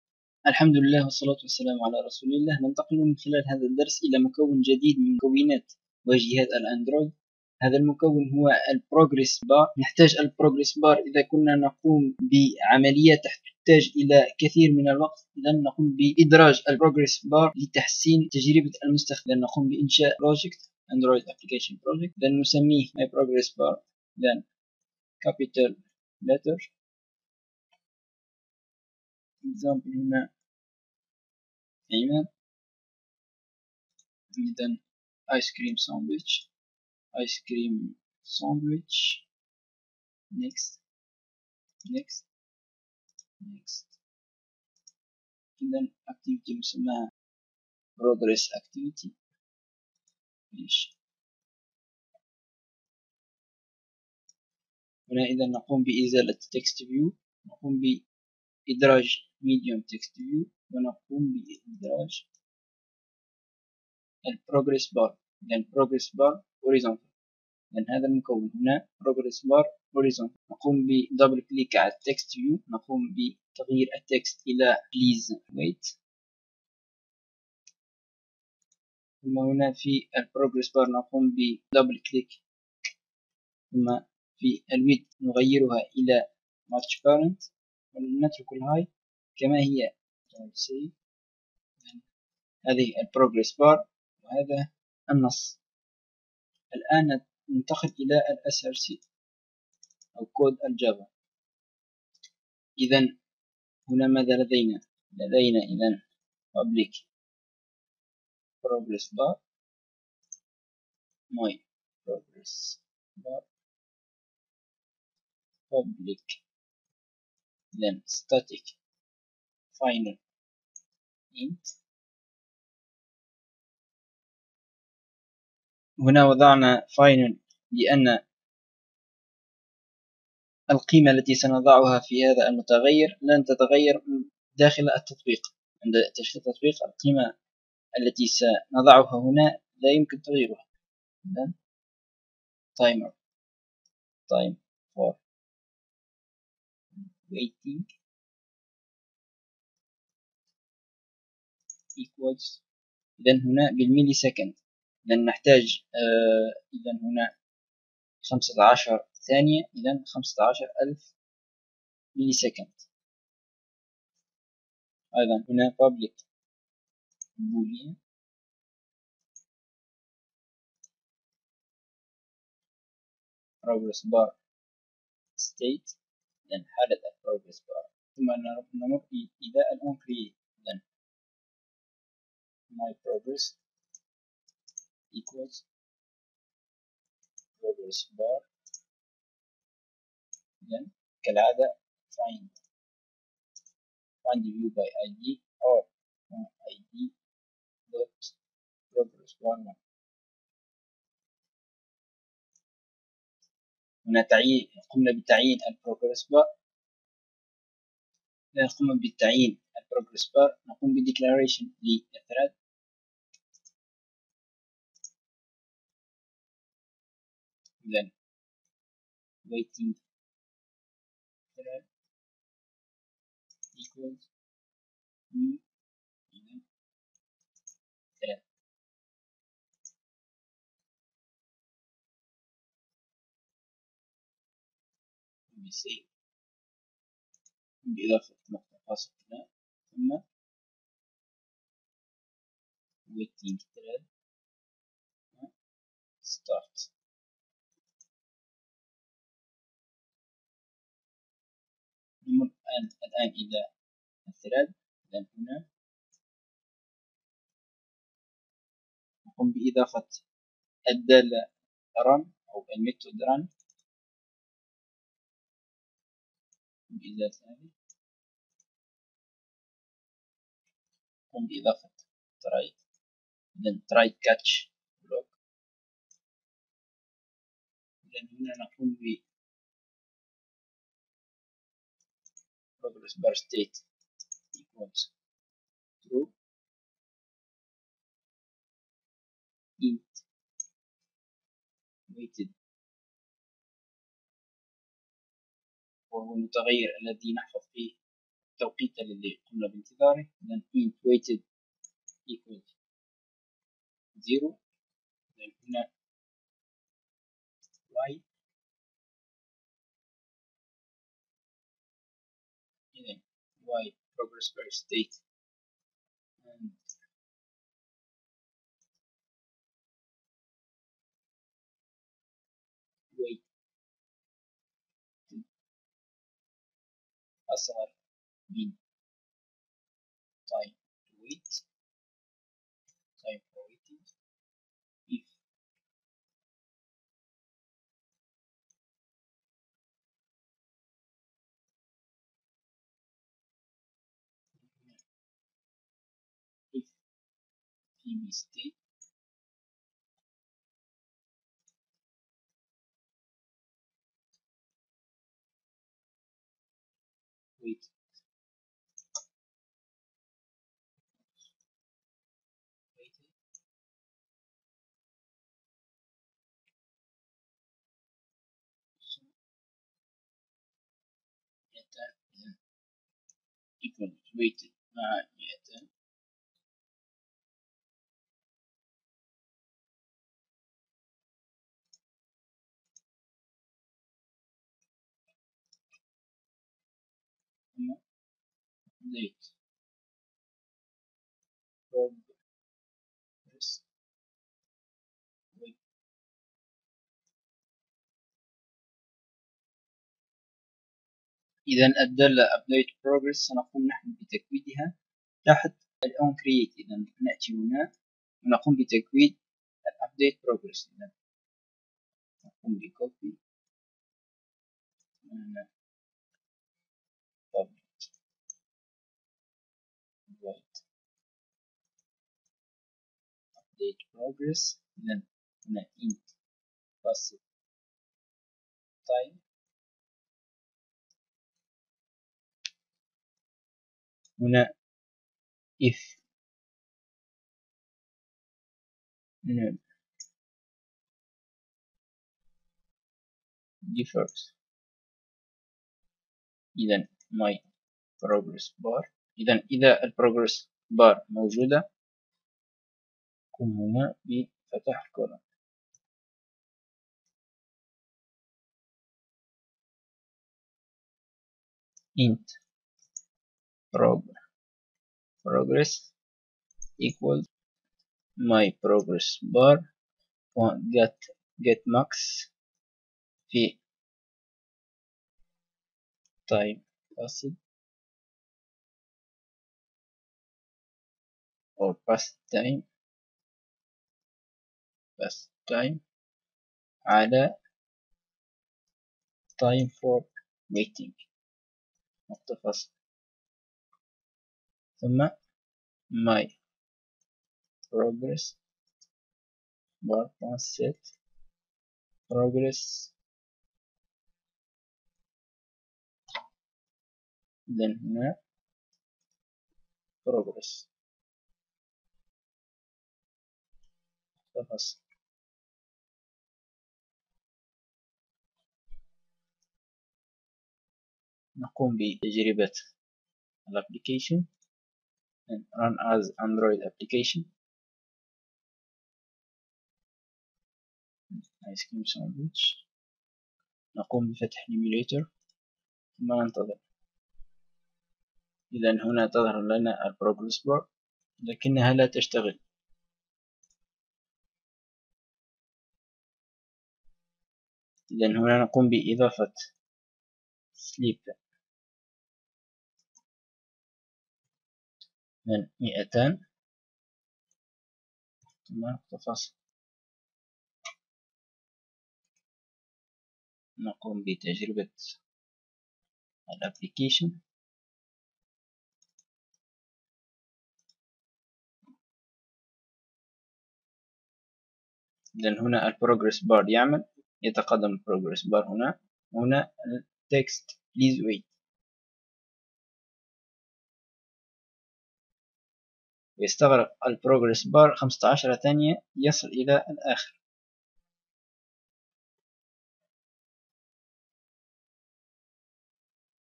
دورة تعليم برمجة تطبيقات الأندرويد (الدرس الخامس عشر) شرح شريط التقدم ProgressBar و كيفيّة تكويدها
أطرح من خلال هذه المدوّنه دورة في تعليم برمجة تطبيقات الأندرويد تضمّ مجموعة من المقالات و الدّروس و الشروحات النّصّيّه و الفيديوهات.

نقوم من خلال هذا الدرس بشرح شريط التقدم ProgressBar و كيفيّة تكويدها .